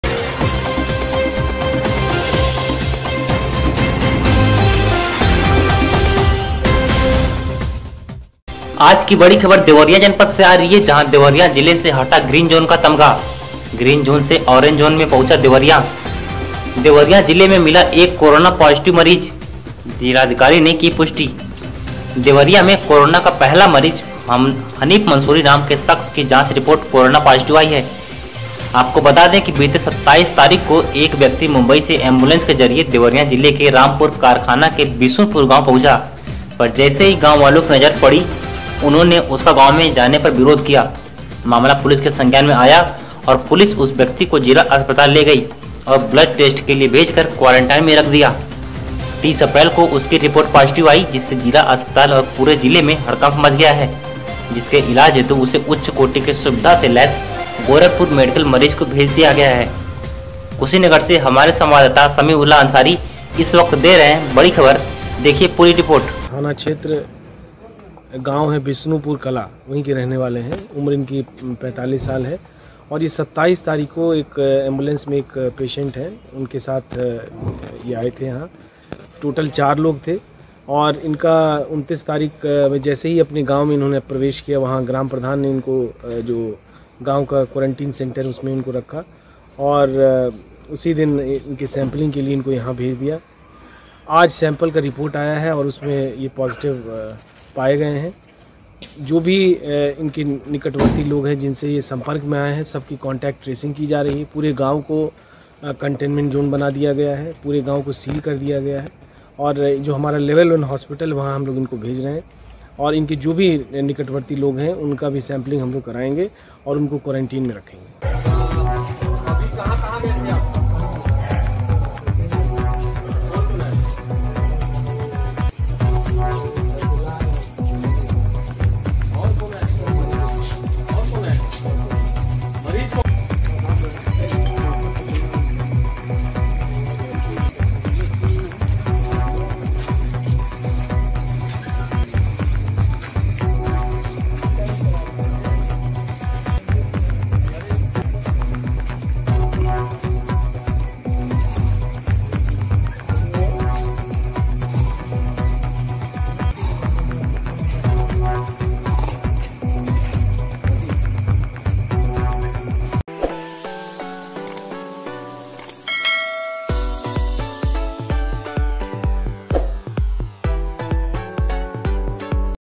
देवरिया में मिला पहला कोरोना मरीज, हटा ग्रीन जोन का तमगा
बिते 27 तारीख को एक व्यक्ति मुंबई से एम्बुलेंस के जरिए देवरिया जिले के रामपुर कारखाना के विशुनपुर गांव  पहुंचा  पर जैसे ही गांव वालों की नजर पड़ी उन्होंने उसका गांव में जाने पर विरोध किया.. मामला पुलिस के संज्ञान में आया और पुलिस उस व्यक्ति को जिला अस्पताल ले गई और ब्लड टेस्ट के लिए भेजकर क्वारेंटाईन में रख दिया 30 अप्रैल को उसकी रिपोर्ट पॉजिटिव आईं जिससे जिला अस्पताल और पूरे जिले में हड़कंप मच गया